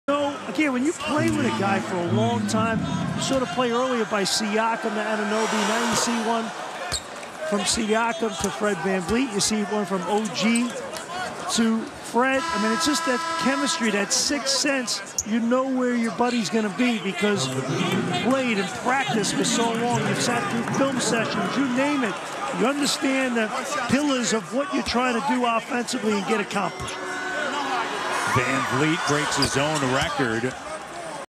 Kelly Oubre Jr YAPPING at Chris Boucher at the Free Throw Line
Kelly Oubre Jr and Chris Boucher beef!? Toronto Raptors vs Charlotte Hornets highlights. Monday February 7, 2022 (02/7/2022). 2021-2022 NBA Regular Season Highlights.